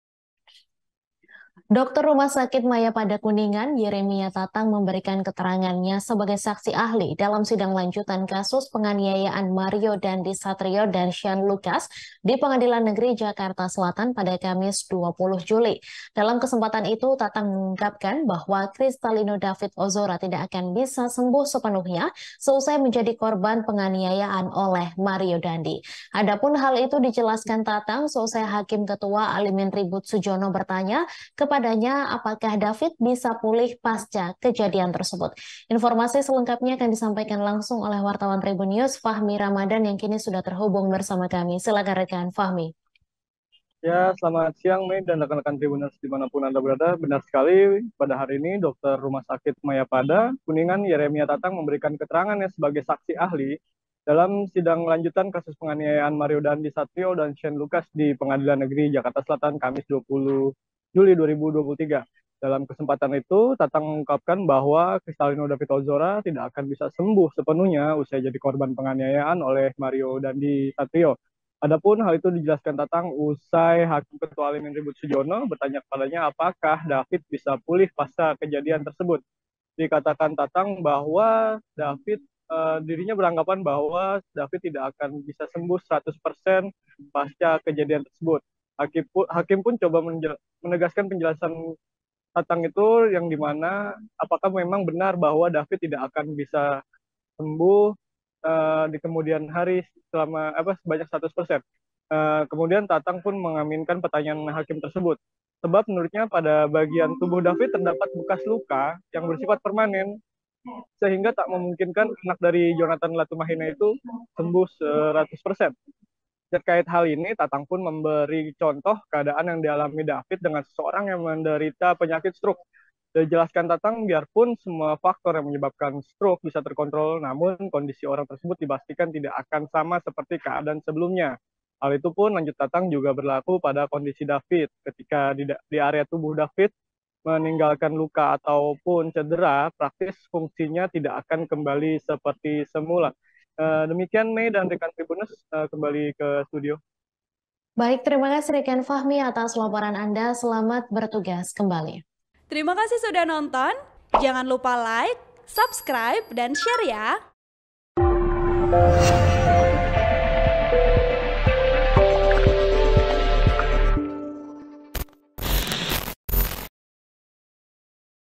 Dokter RS Mayapada Kuningan Nyatakan David Ozora Tidak Bisa Sembuh Sepenuhnya usai Penganiayaan
TRIBUN-VIDEO.COM - Dokter Rumah Sakit Mayapada Kuningan, Yeremia Tatang memberikan keteranganya sebagai saksi ahli dalam sidang lanjutan kasus penganiayaan Mario Dandy Satriyo dan Shane Lukas di Pengadilan Negeri Jakarta Selatan, Kamis (20/7/2023).

Dalam kesempatan itu Tatang mengungkapkan bahwa Crystalino David Ozora tidak akan bisa sembuh sepenuhnya usai jadi korban penganiayaan oleh Mario Dandy.

Adapun hal itu dijelaskan Tatang usai Hakim Ketua Alimin Ribut Sujono bertanya kepadanya apakah David bisa pulih pasca kejadian tersebut.(*)

Video Editor : Rania